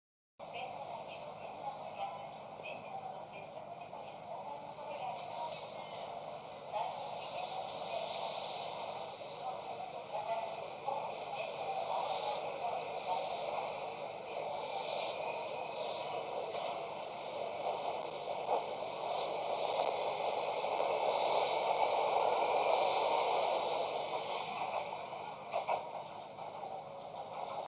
K1007レ特急新開地行き1000系1017Fハチワレ号(現在はノーマル)
撮影日2023年8月6日 神戸三宮駅
この日､8日(火)夕方に大阪梅田駅で線路発煙による車両トラブルで運休しておりましたか現在は元気で運行しています